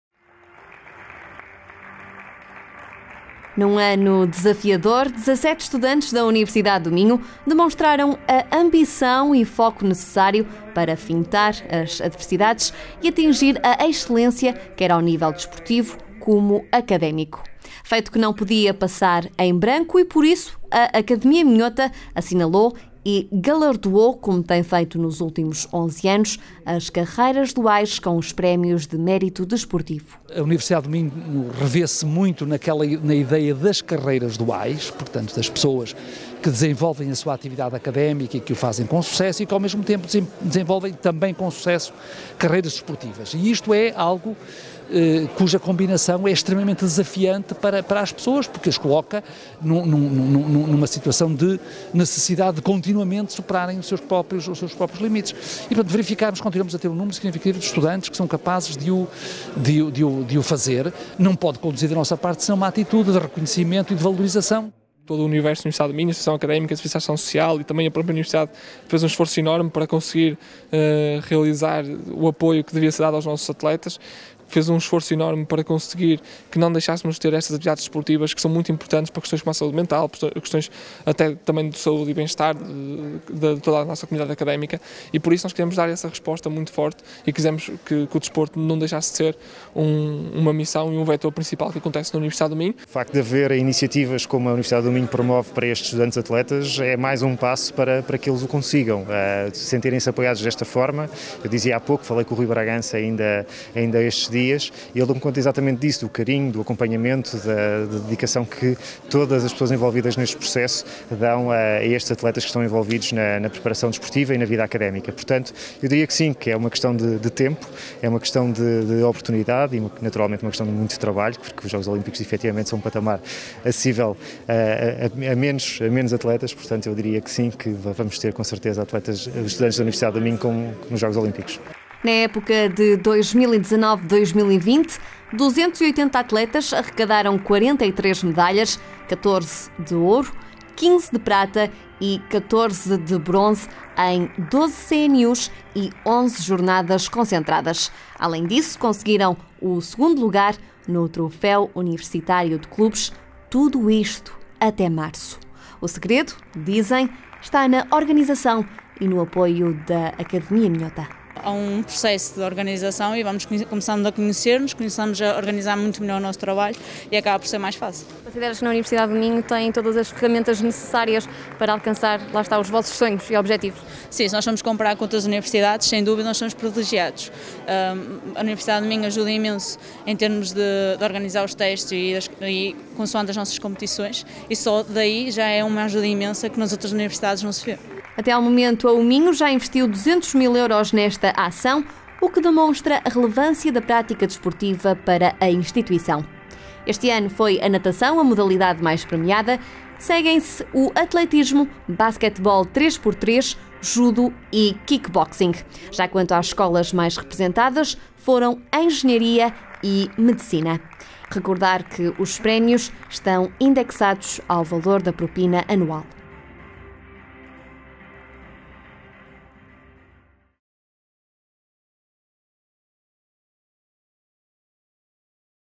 Prémios de Mérito Desportivo 2020
Dezaseis estudantes/atletas da UMinho, verdadeiras referências do desporto universitário, receberam o "Prémio de Mérito Desportivo da UMinho 2020".

O desenvolvimento de carreiras duais de modo a atingir o sucesso académico em paralelo com a excelência desportiva é algo que nos diferencia enquanto Universidade e este prémio é o reconhecimento pelo esforço e dedicação de todos ao longo do último ano letivo.